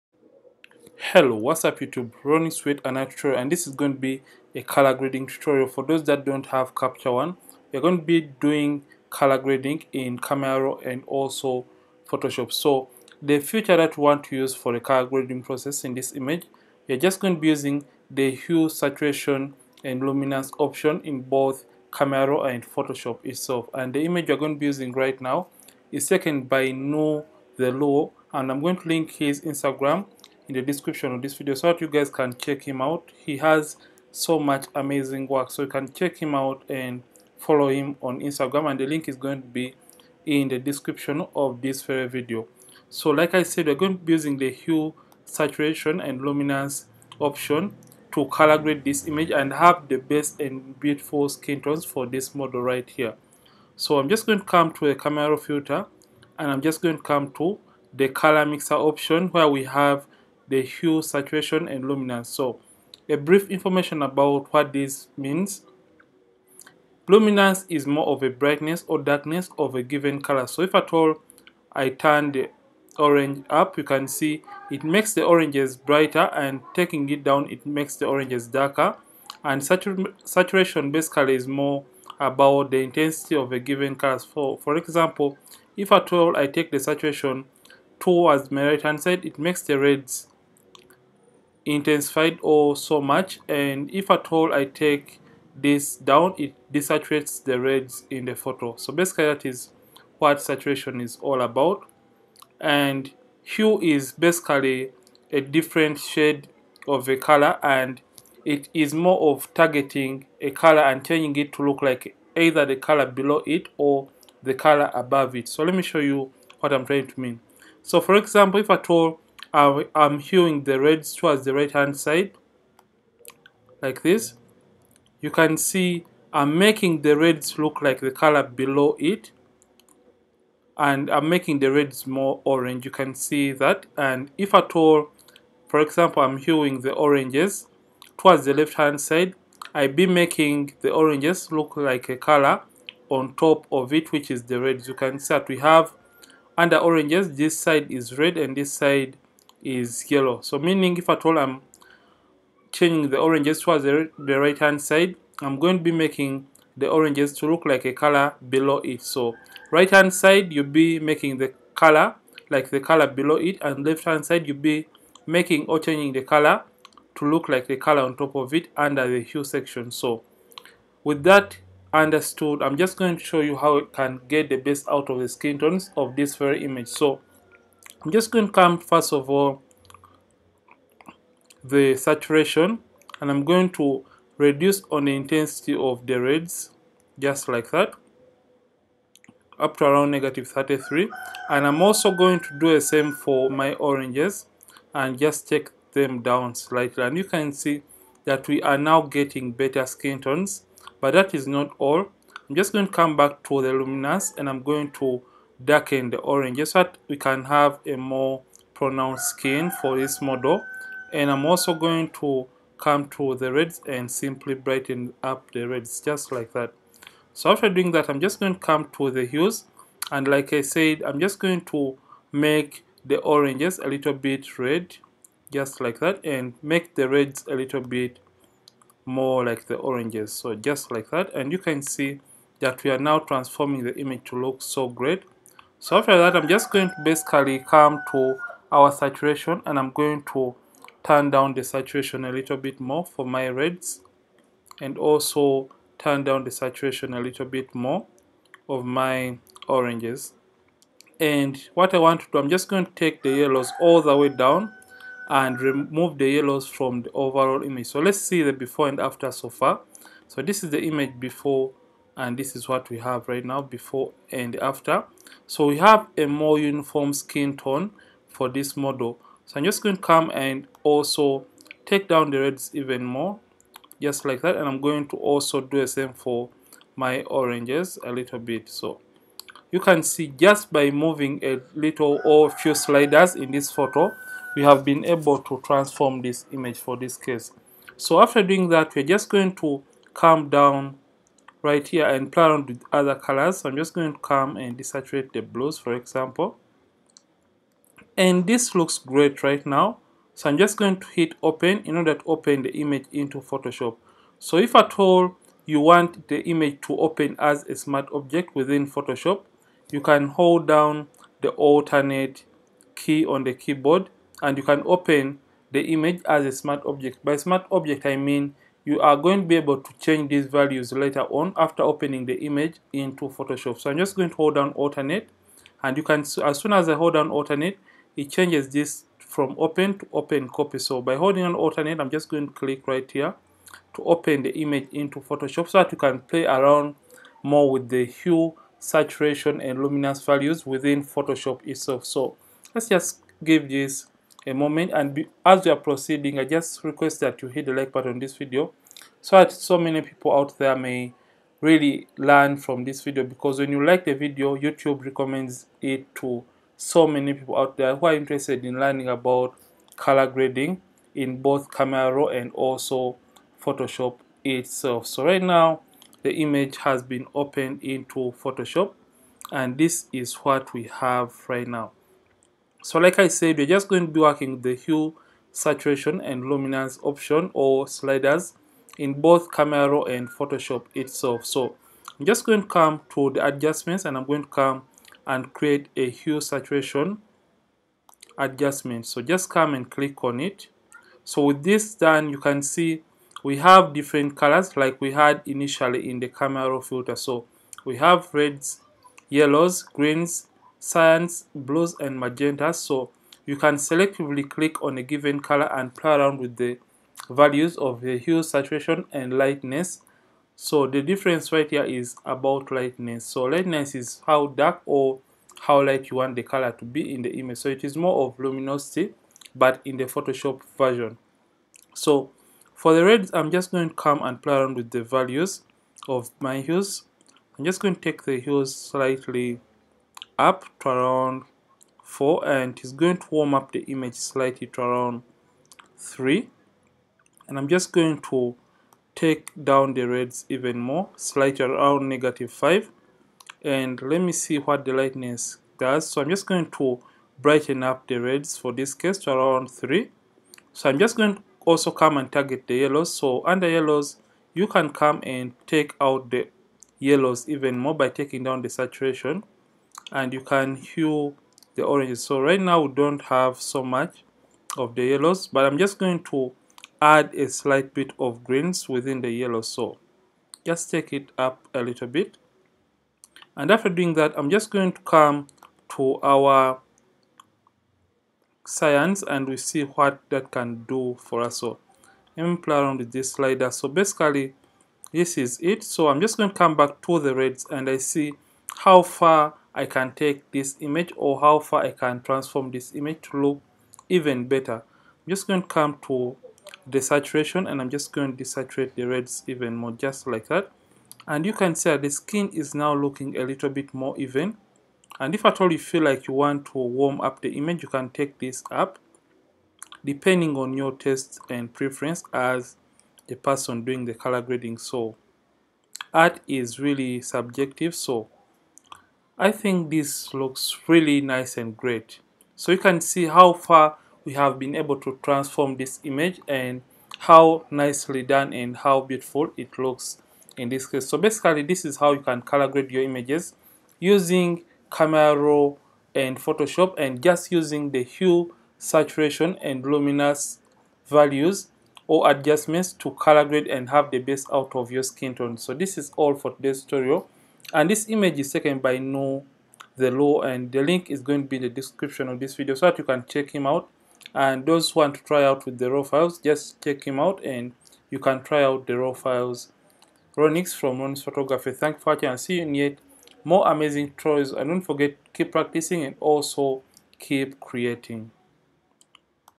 COLOR GRADE SKIN TONES USING HSL IN PHOTOSHOP | Camera Raw & Photoshop Tutorial
Learn the best way to color grade your photos in camera raw and lightroom by first getting to learn everything about the Hue, Saturation and Luminance Sliders. With this knowledge you will be able to color grade with ease in photoshop.